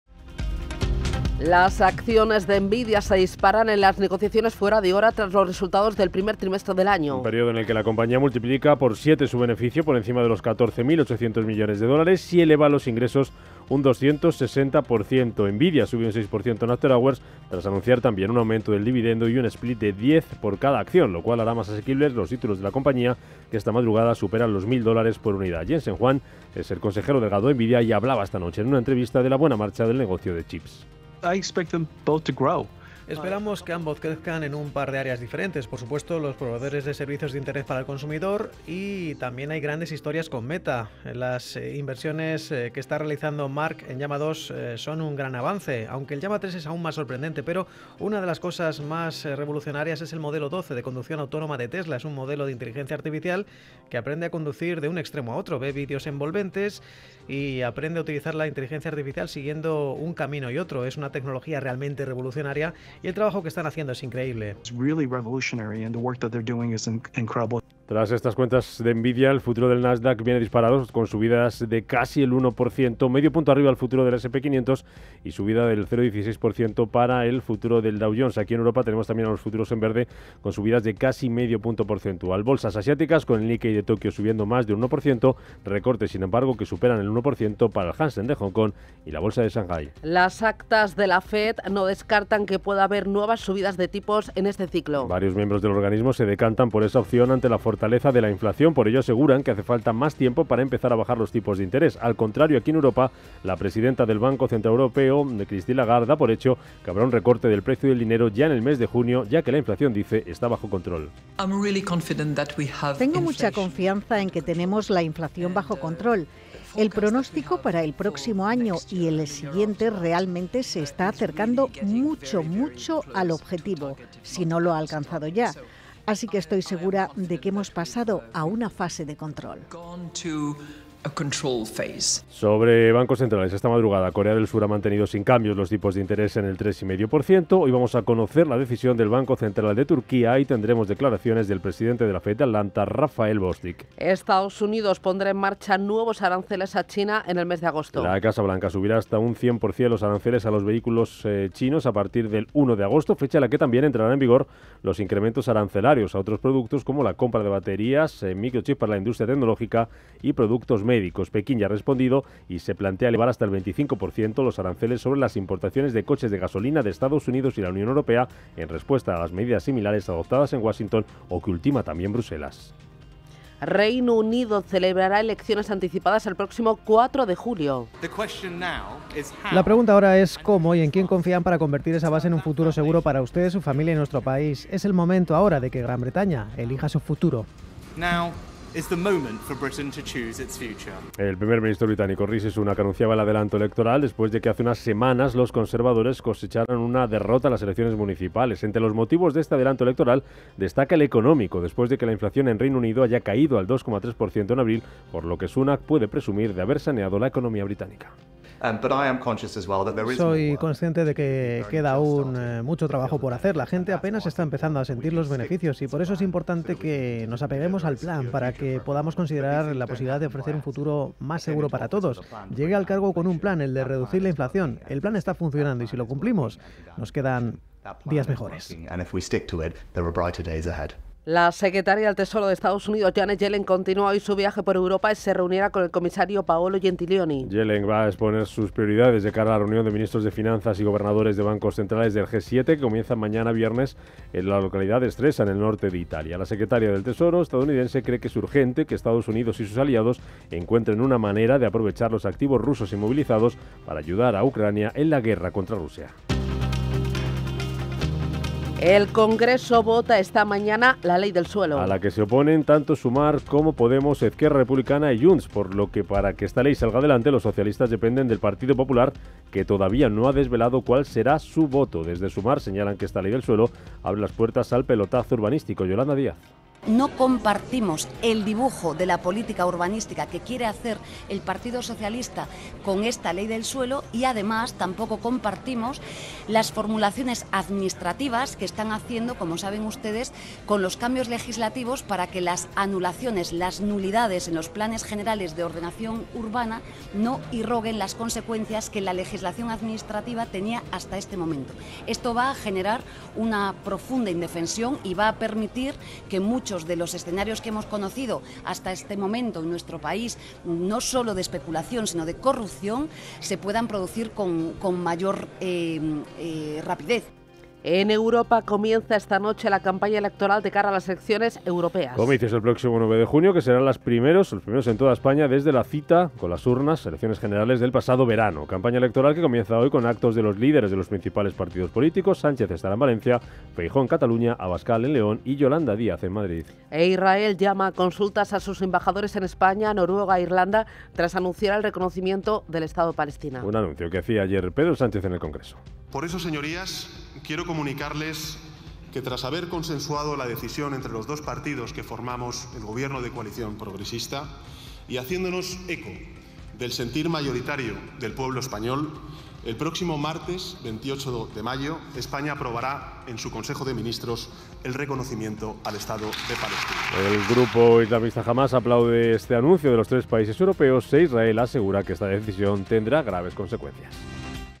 Nvidia rompe expectativas, Reserva Federal en la mira y ley del suelo al descubierto
En este video, analizamos los resultados financieros de Nvidia y su impacto en el mercado. También discutimos las dudas e incertidumbres que rodean a la Reserva Federal y su política monetaria actual. Además, exploramos la nueva Ley del Suelo y sus posibles efectos en el sector inmobiliario.